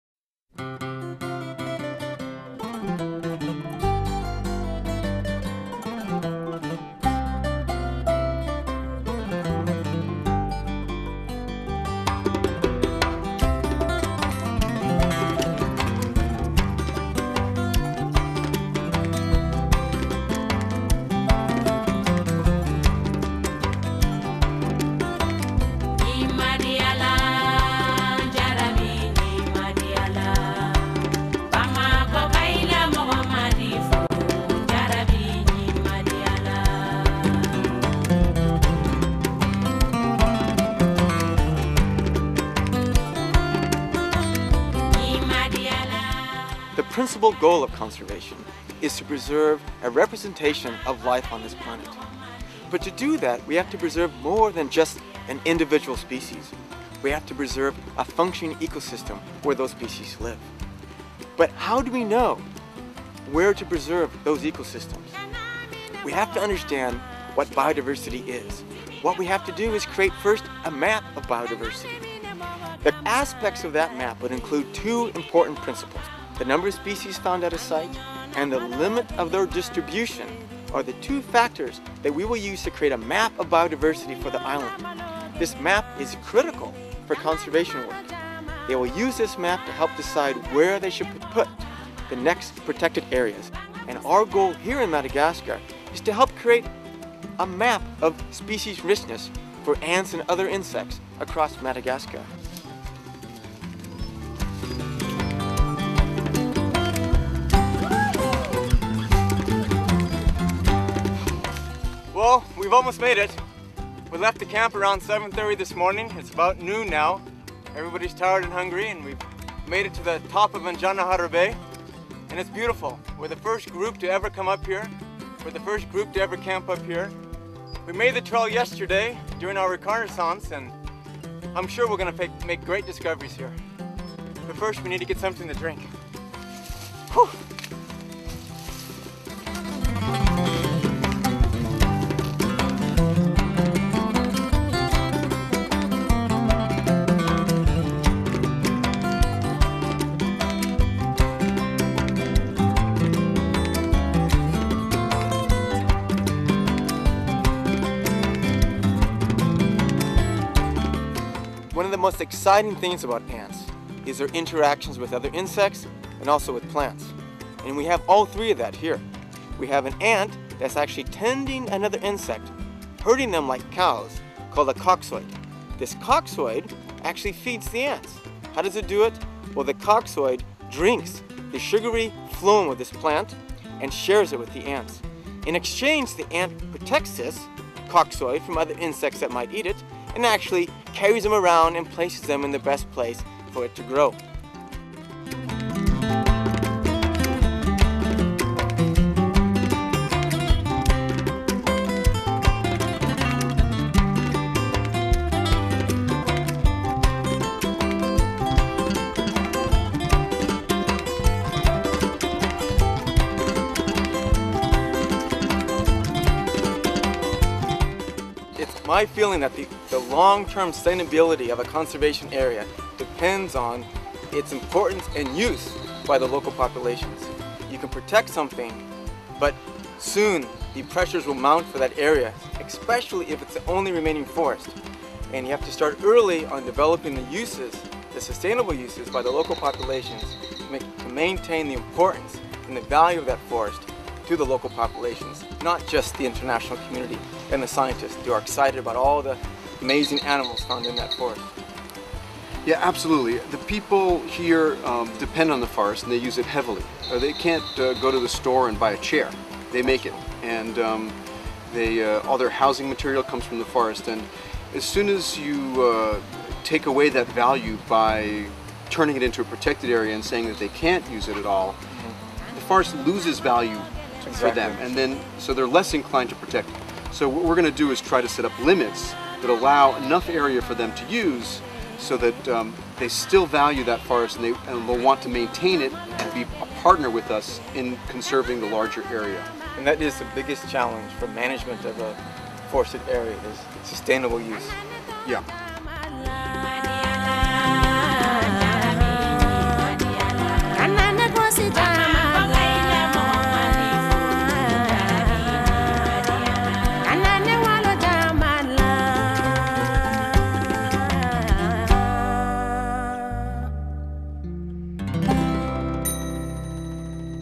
A Map to Save an Island
In 2003 Chris Baldwin of Shoulder High Productions followed Dr. Brian Fisher Entomologist of the California Academy of Sciences to Madagascar.  Dr Fisher taught us how studying the ants of this island provides a great insight into the conservation goals of the whole planet.